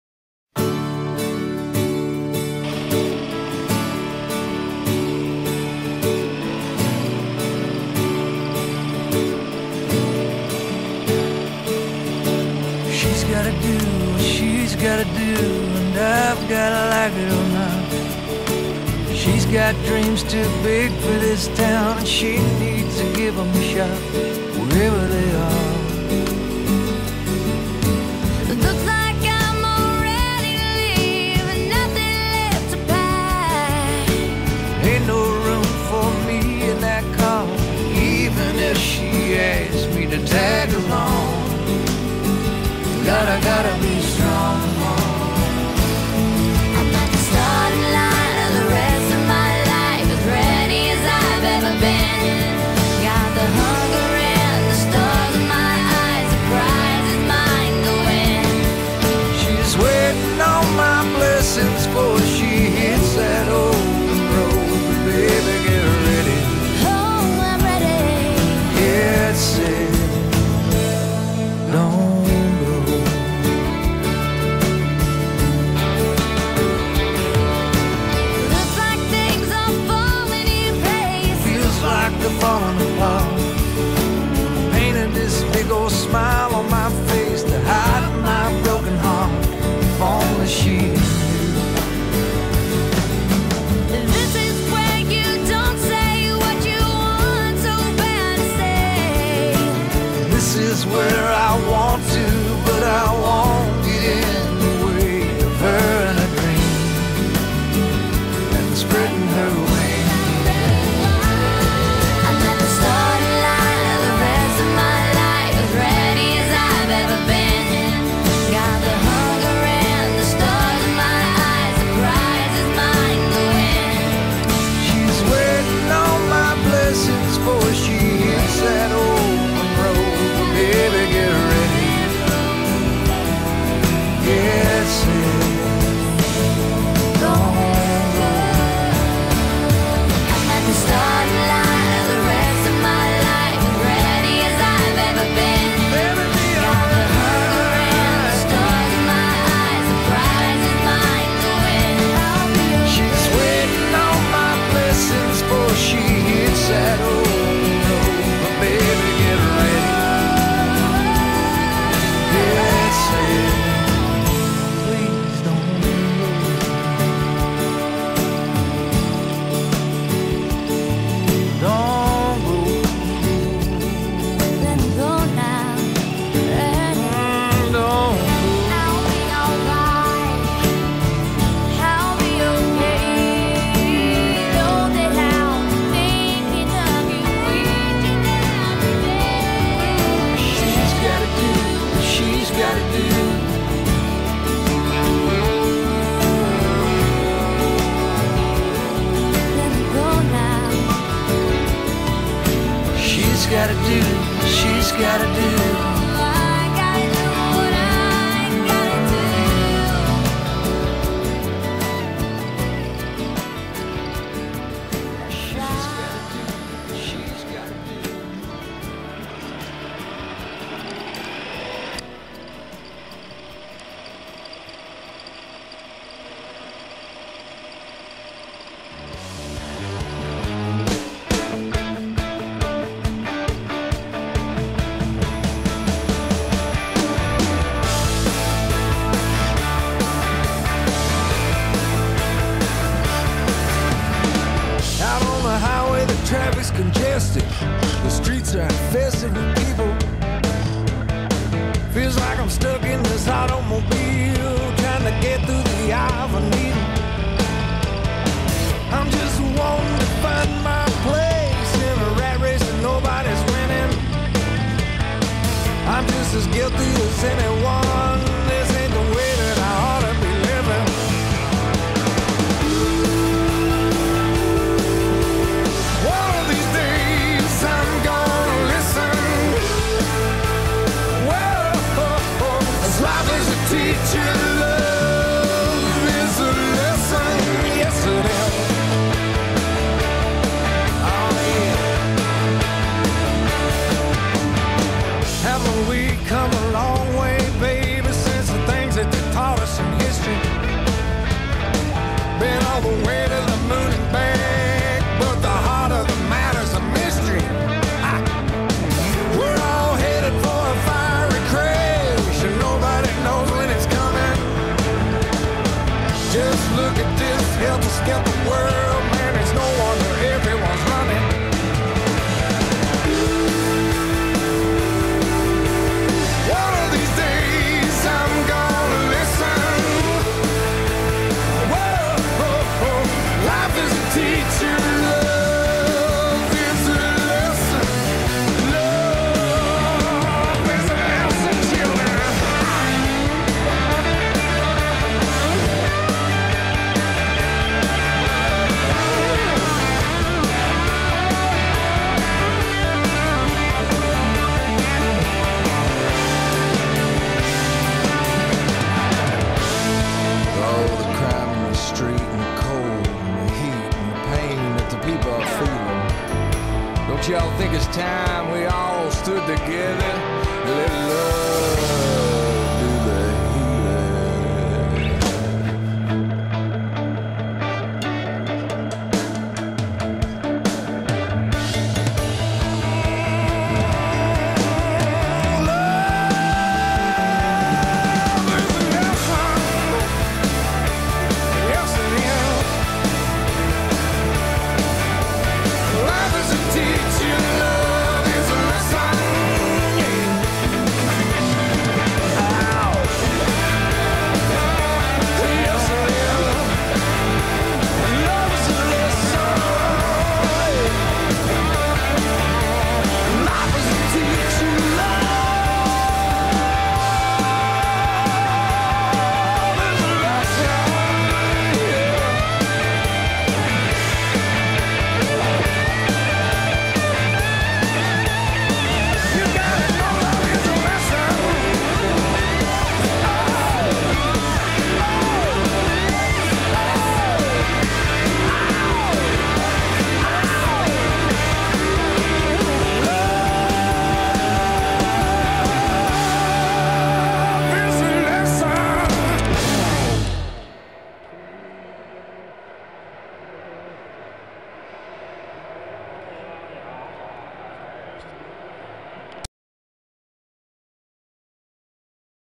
Darwen Truck Fest Darwen 2010.wmv
Mini cam on Jeff's Tamiya Knight Hauler. The 1st Truck Fest at Darwen